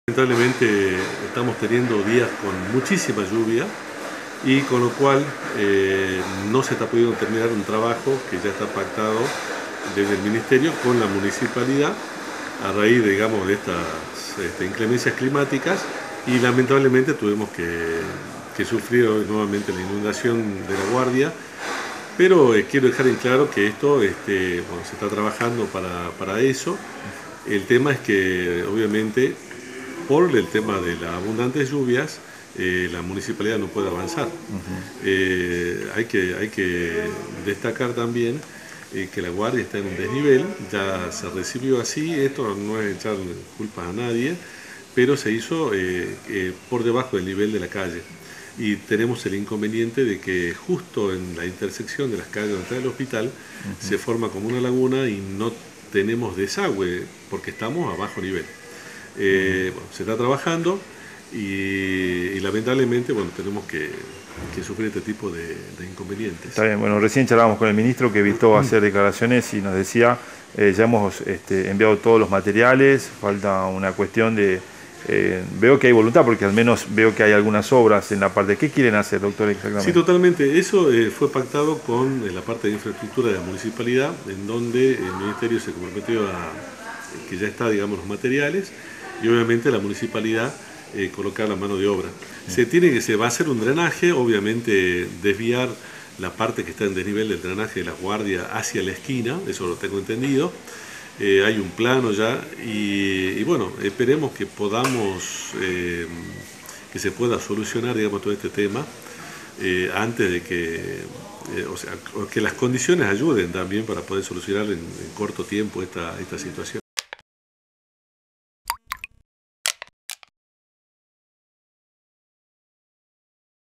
Temporal en las Yungas se inundó la guardia del Hospital Orias en Libertador
El centro de atención está en desnivel y no cuenta con desagües.